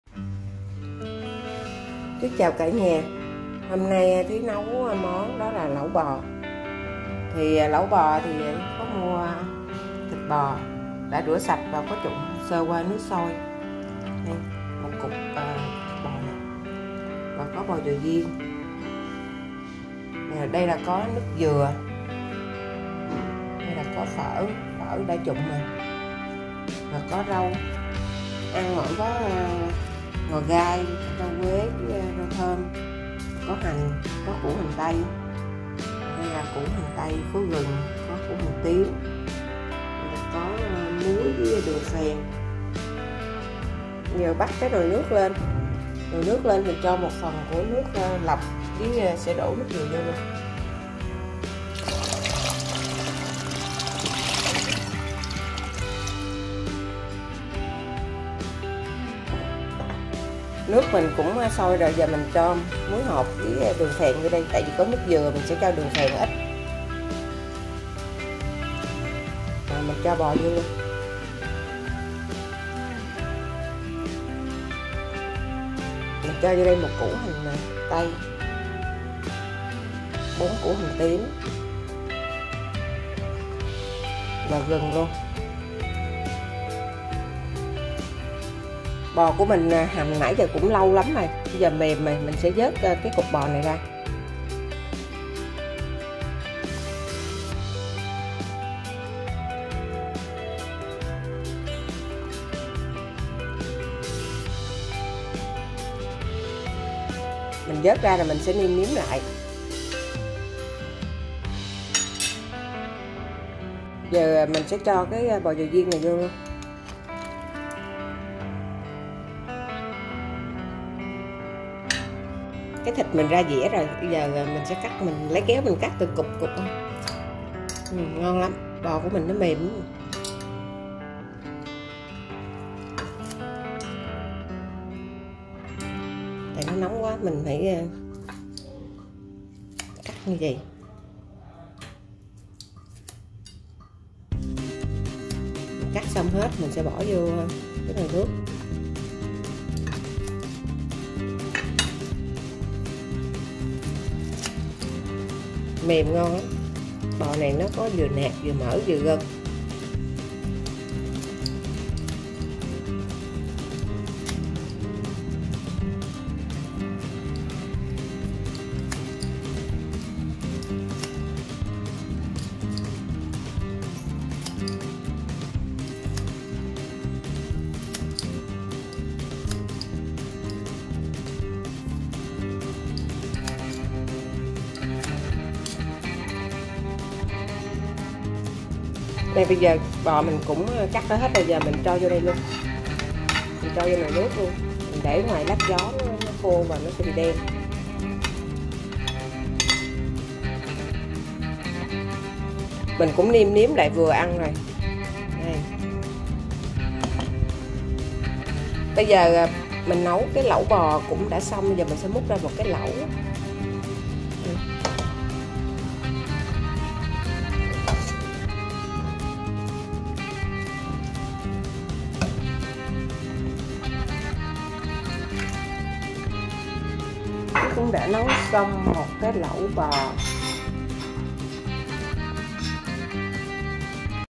CÁCH NẤU LẨU BÒ ĐƠN GIẢN MÀ RẤT NGON
Món lẩu bò cách nấu đơn giản.
Mà thơm ngon
Cả nhà vô bếp nấu món lẩu bò nha .